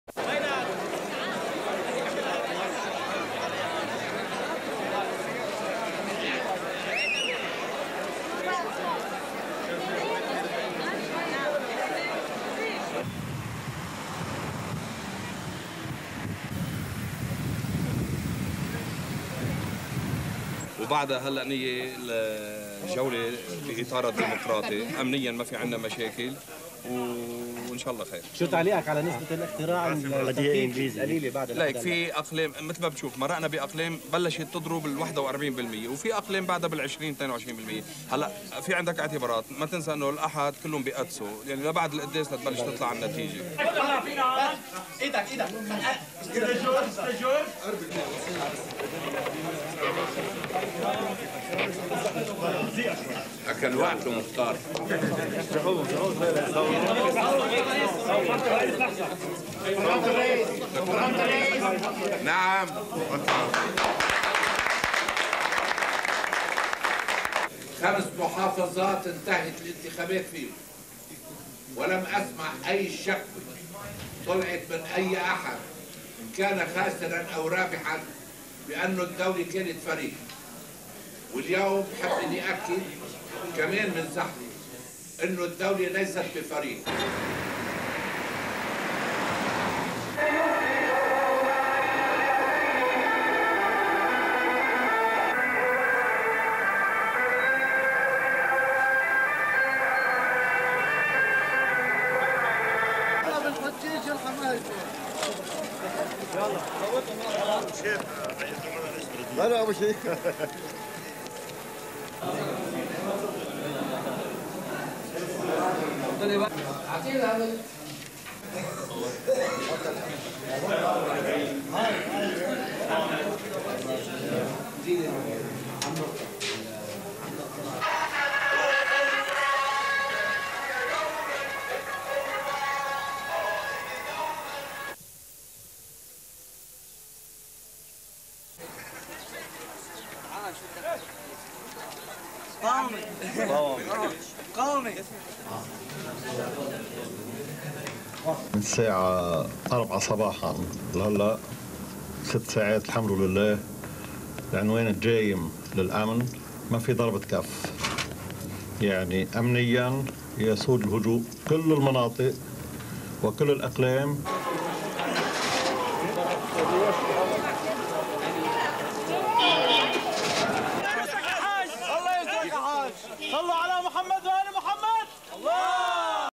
Lebanon - Municipal elections
(14 Jun 1998) T/I:   10:38:49

The Shiite Moslem fundamentalist Hezbollah went into a final showdown with tribal chiefs in eastern Lebanon on Sunday (14/6) in the fourth and last phase of the country's first municipal elections in 35 years.
While Hezbollah seeks to appease fears with its "List of National Coexistence," Baalbek's powerful tribal chiefs formed a rival coalition ticket with several parties including Hezbollah's Shiite arch-rival, the Amal.
image and win back tourists to its ancient Roman monuments.

SHOWS:

ZAHLE + BAALBEK, LEBANON. 14-06:

ZAHLE:
Polling station in Zahle,
people throwing lists in the air,
people gathering in front of polling station;
WS Street in Zahle - the hometown of Lebanese President Elias Hrawi, security on the streets;
CU Georges Hrawi, son of Lebanese president Elias Hrawi who is backing the candidates opposing his father's group;
SOT (Arabic) Georges Hrawi saying that the atmosphere is democratic and the participation is 41 percent in some areas;
Main opposition MP Elie Skaff voting in Zahle;
Ws President Hrawi voting in Zahle;
Brief statement from President Hrawi saying in Arabic that the government is not intefering in the Municipal elections.

BAALBEK:
Street scene in Baalbeck, a stronghold for  Hezbollah;
Baalbeck residents voting,
various of hezbollah cars with microphones calling on voters to vote, various of cars touring the streets,
woman in a wheelchair coming to vote;
Hezbollah candidate, Hussein Raad going to vote;
Hezbollah representatives of Hezbollah wearing yellow hats with Hezbollah slogans;
Various of people voting,
GV int polling station, crowded street,
Hezbollah representatives canvassing;
GV people casting ballots,
ws Hezbollah car touring streets of Baalbeck;
VS Interior Minister, Michel al-Murr;
SOT (Arabic) Michelal-Murr: "Security is implemented and there were no breaches of security of any kind at today's polls"
VS polling station with security guards,
Hezbollah supporters cheer as one of their supporters enters polling station to vote for their candiate.

2.56